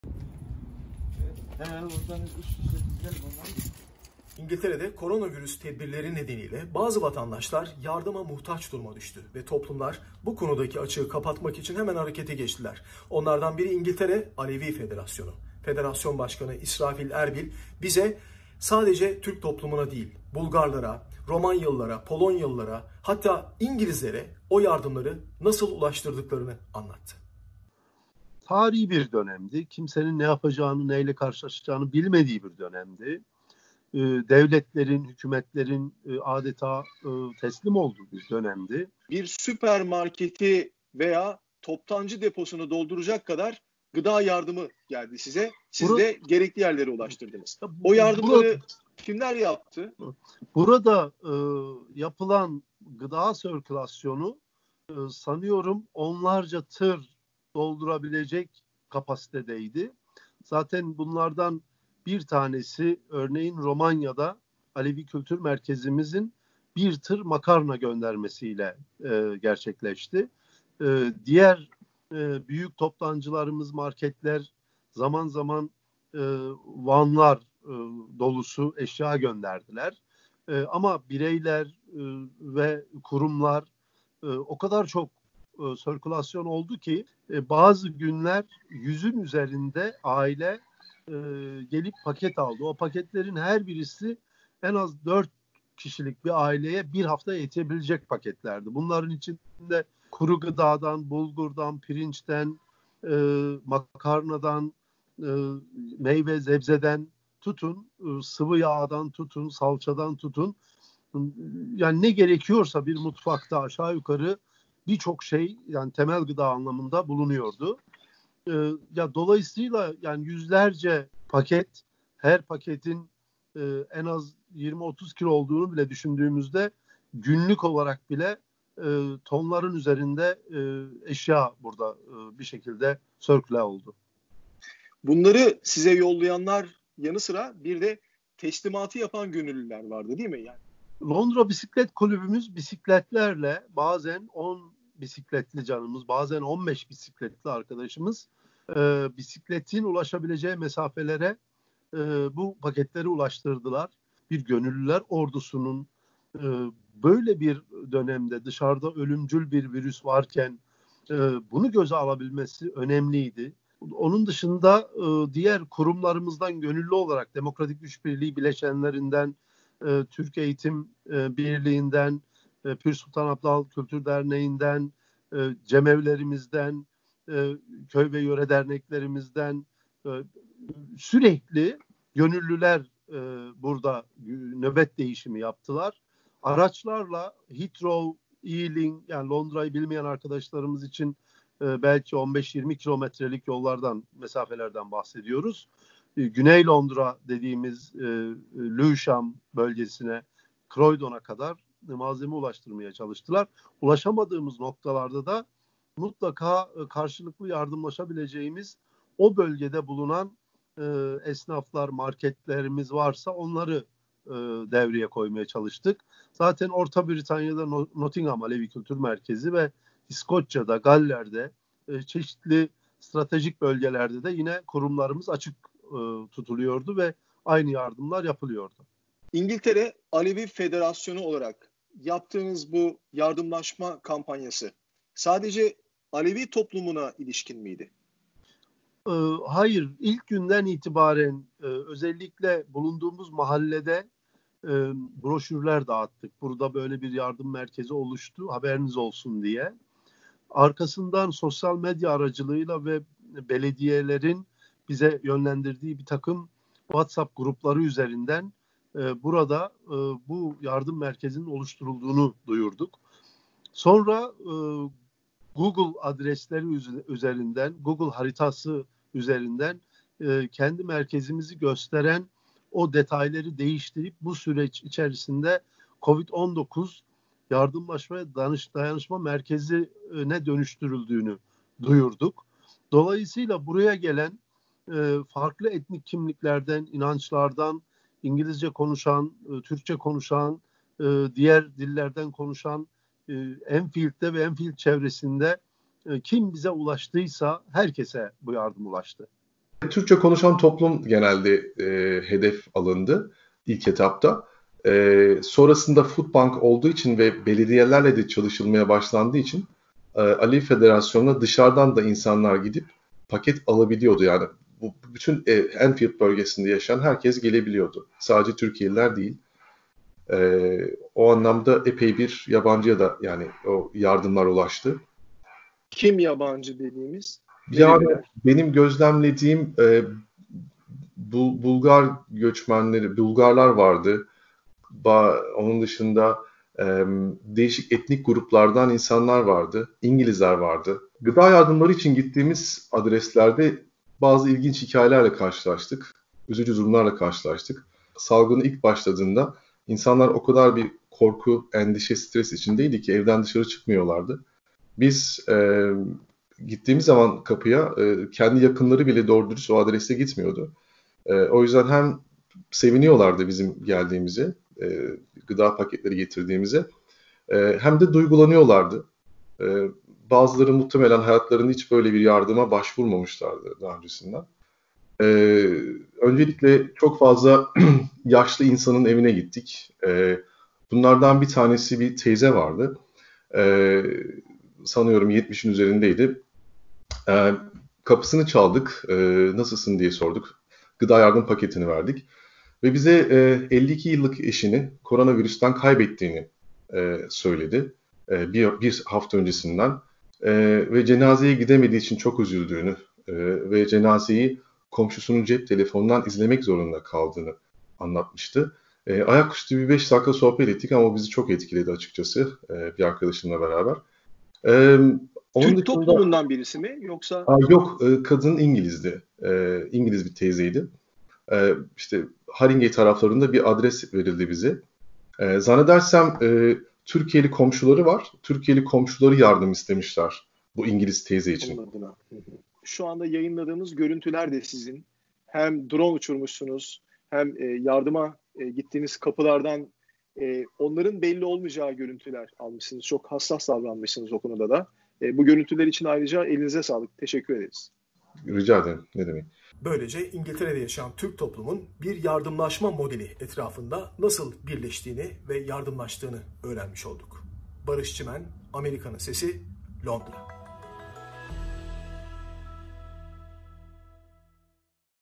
İngiltere Alevi Federasyonu’ndan İhtiyacı Olanlara Corona Yardımı
İngiltere’de, Corona virüs tedbirleri nedeniyle bazı vatandaşlar yardıma muhtaç duruma hale geldi. Toplumlar bu konudaki açığı kapatmak için hemen harekete geçti. Onlardan biri İngiltere Alevi Federasyonu